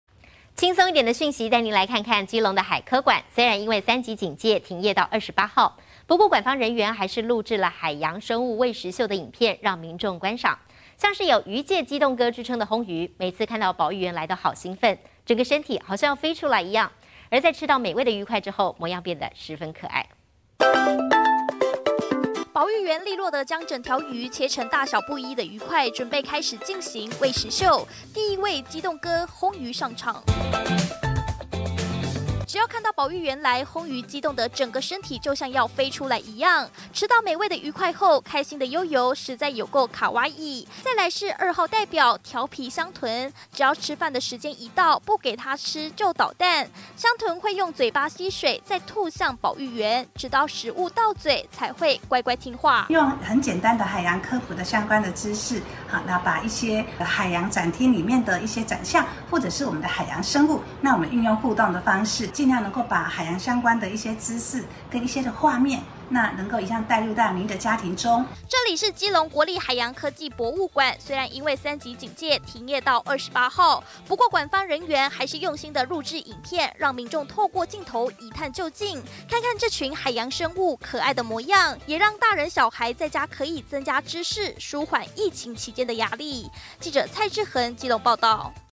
美味魚塊在前 "魚界激動哥"急探水面大口吞
基隆的海科館，雖然因為三級警戒，停業到28號，不過館方人員，還是錄製海洋生物餵食秀的影片，讓民眾觀看，像是有魚界激動哥之稱的魟魚，每次看到保育員來都好興奮，整個身體就像要飛出來一樣，吃到美味魚塊後，模樣變得十分可愛。